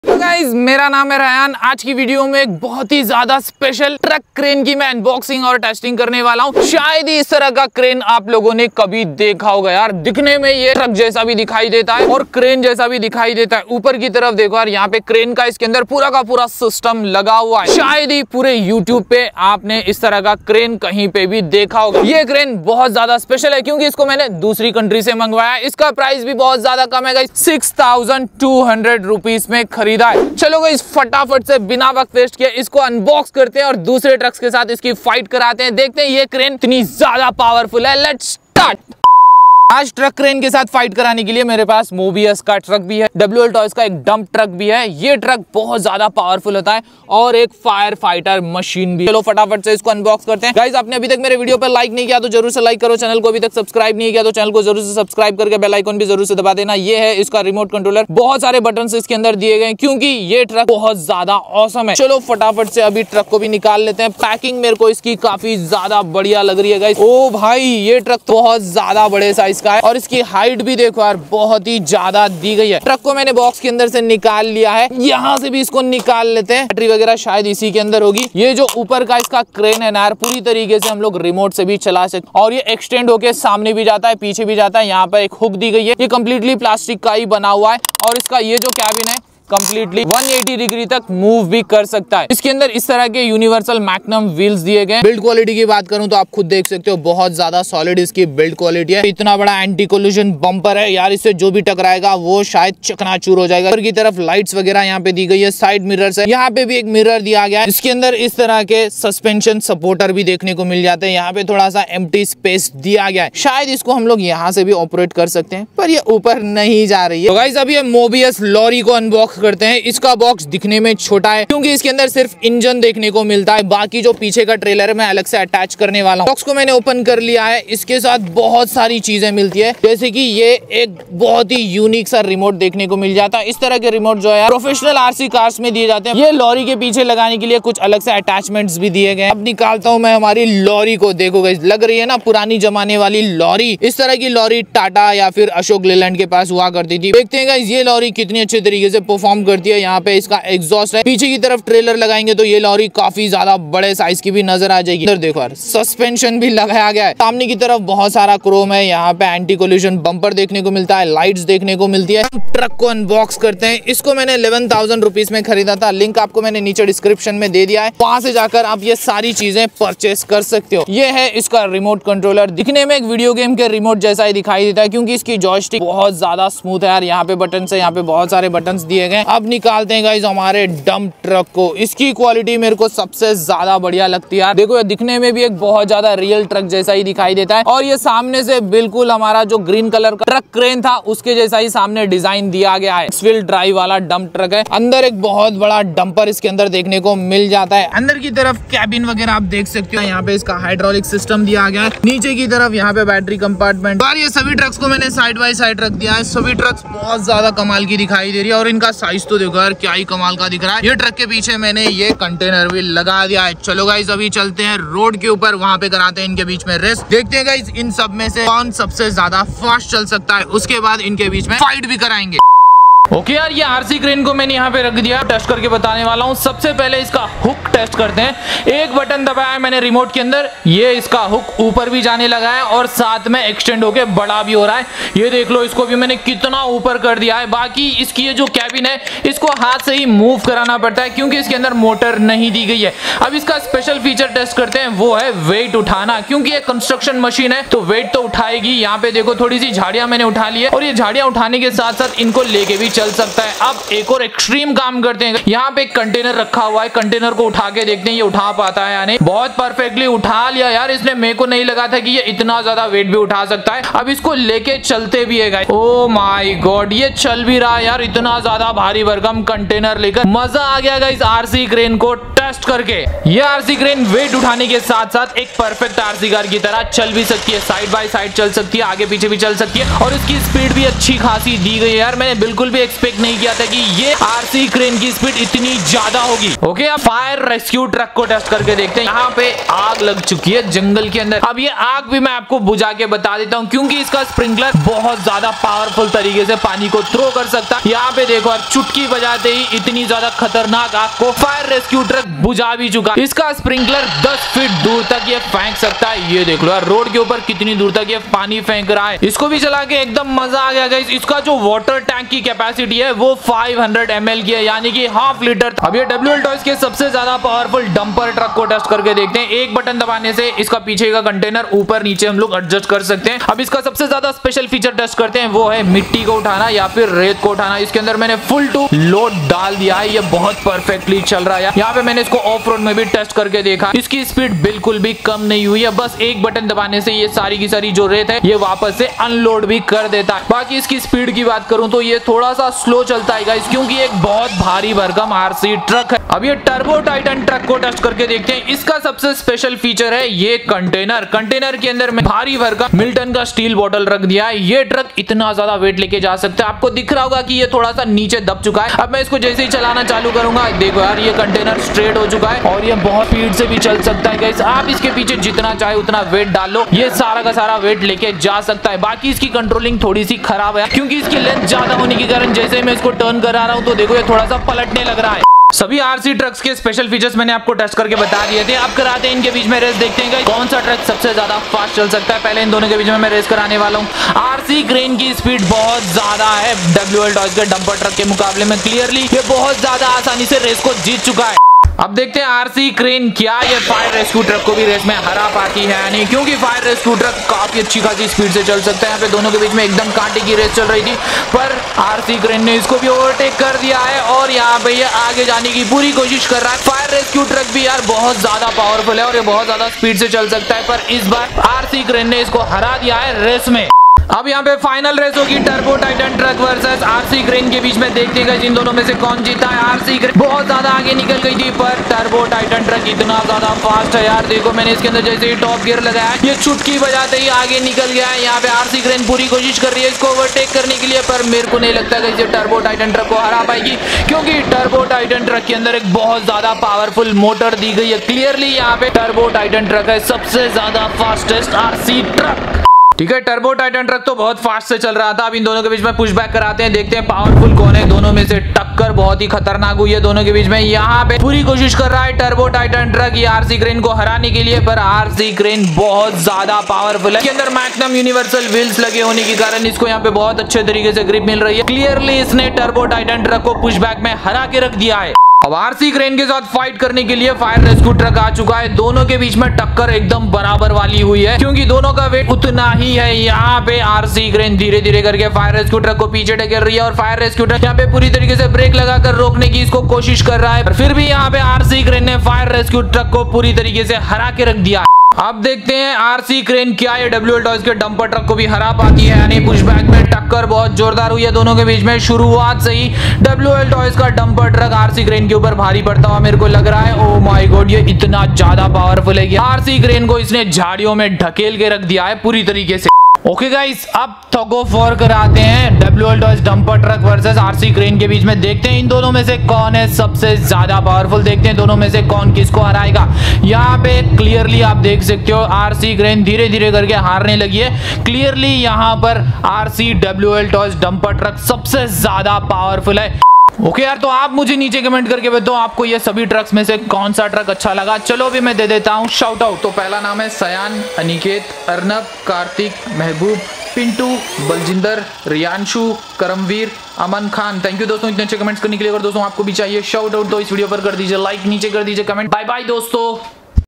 RC Modified Jumbo High Speed Truck Unboxing & Testing - Chatpat toy tv
Dosto aaj ki video mai maine unbox aur test kiya hai rc high speed lifter truck ko yeh bohat jayda weight utha sakta hai iske andar moving hook hai iski top speed 30 kmph hai

Buy Links
RC fasest loader Truck big size Turbo titan Truck